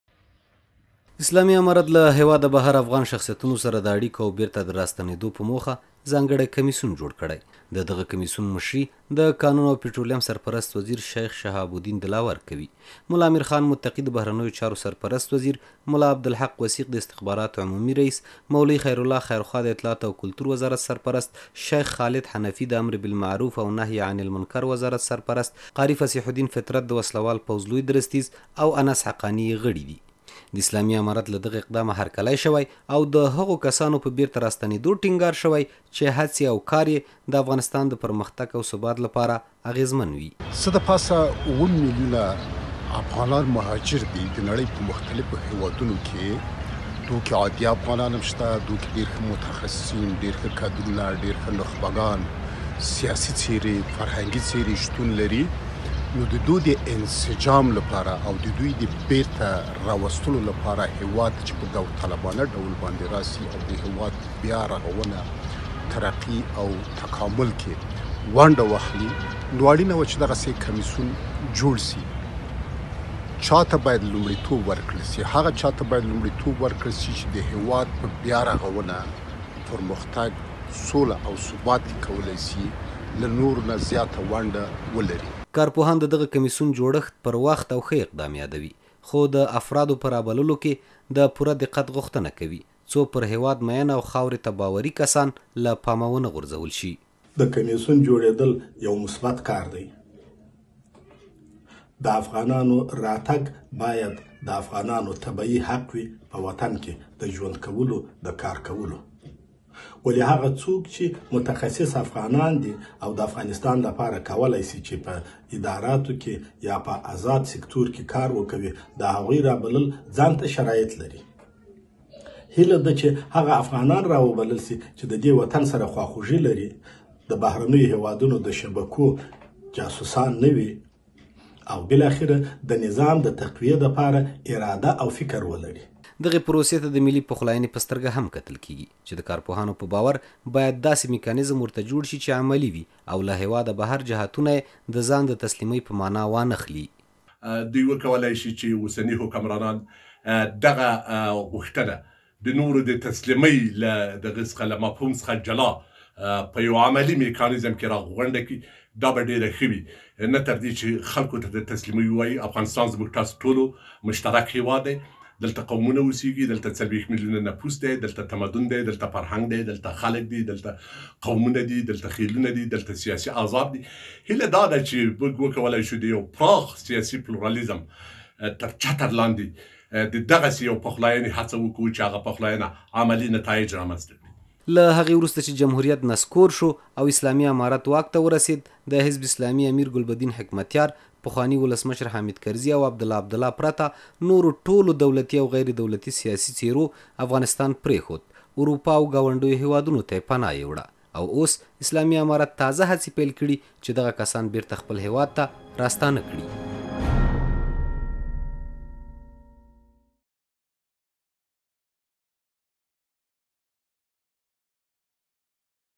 له هیواد بهر افغان شخصیتونو سره داړیکو او بیرته راستنیدو په موخه جوړ شوی کمیسیون پرځای اقدام یادیږي.
غوښتنه  داده چې باید ټول هغه کسان بیرته  راوبلل شي چې دهیواد په بیا رغونه او دایمي ثبات لپاره اغیزمن تمامیدای شي.
کارپوهان وايي کمېسیون دې دهغو کسانو د بیا راتګ هڅه نه کوي چې له هیواد سره خواخوږي نه لري او دنورو هیوادونو د شبکو لپاره کار کوي